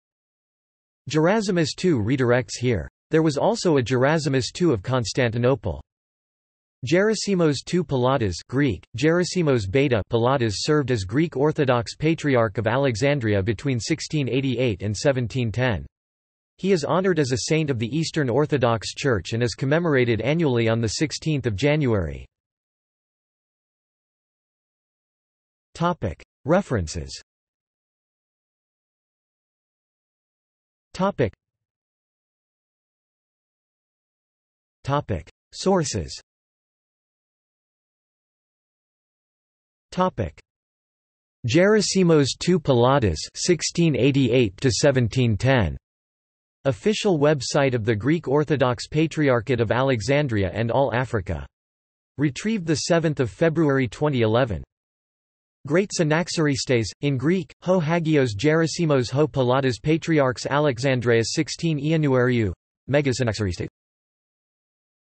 Gerasimos Palladas | Wikipedia audio article
This is an audio version of the Wikipedia Article:
Gerasimos Palladas

SUMMARY
Gerasimus II redirects here. There was also a Gerasimus II of Constantinople.
Gerasimos II Palladas (Greek: Γεράσιμος Β' Παλλαδάς) served as Greek Orthodox Patriarch of Alexandria between 1688 and 1710. He is honoured as a saint of the Eastern Orthodox Church and is commemorated annually on 16 January.